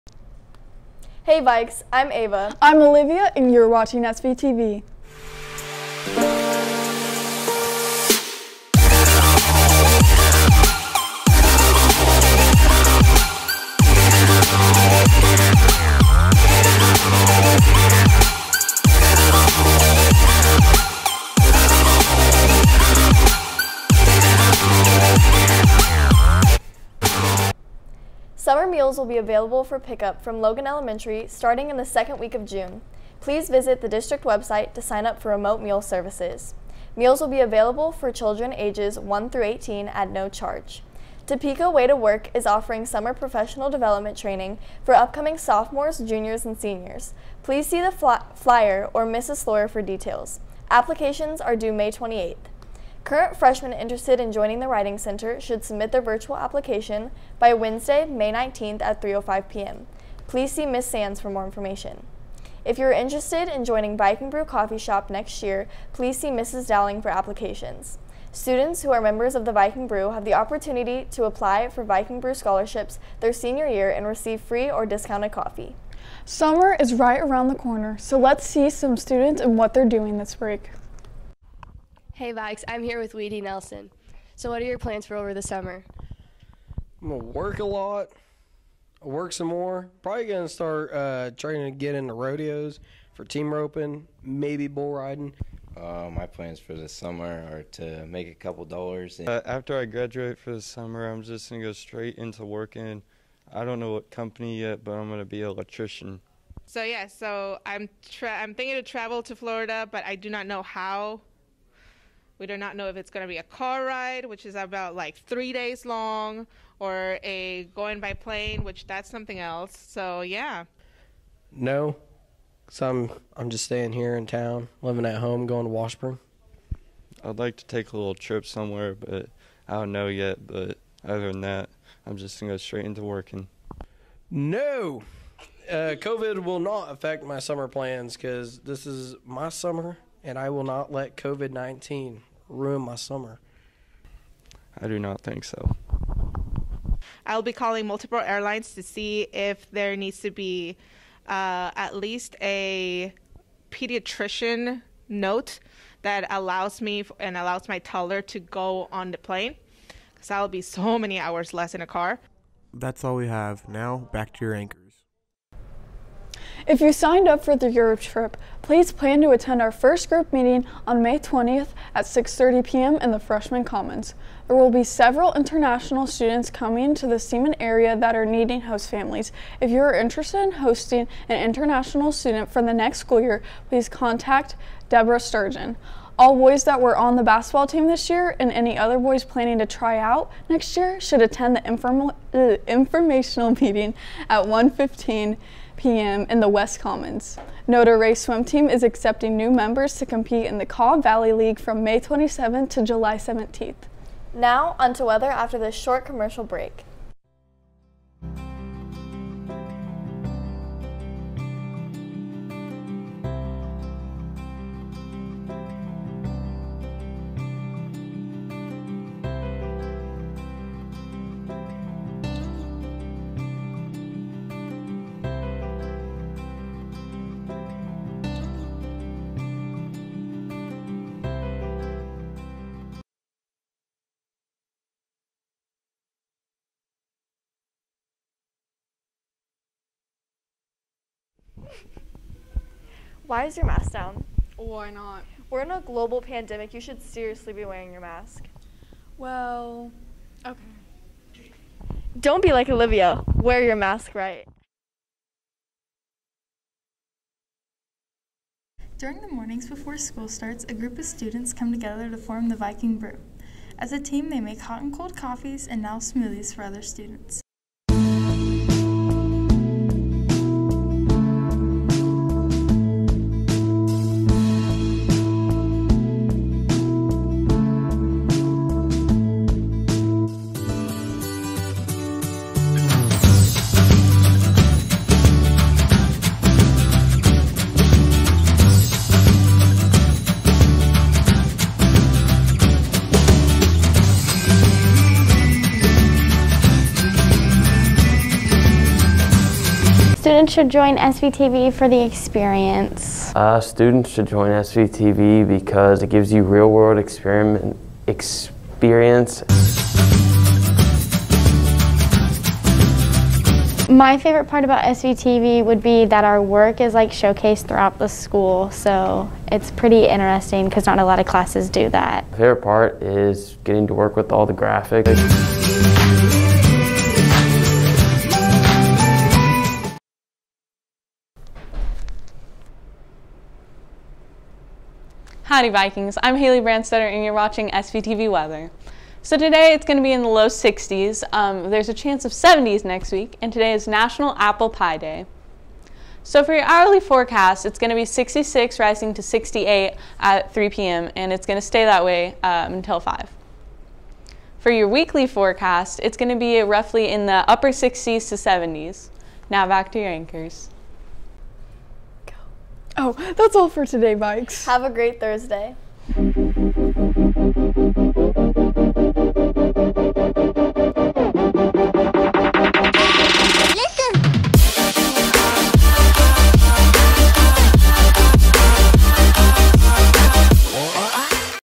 5-13-2021 SVTV Thursday Report
Stories: Kiernan Meredith and Terra Mc Nutt
Anchors: Ava Laird and Olivia Pekarek
Weather: Haley Branstetter
Director: Landon Stuke
Ads:Madyson Hardesty and Makena Schuler; Ava Laird and Olivia Pekarek; Kaidence Heston and Alexis Dultmeier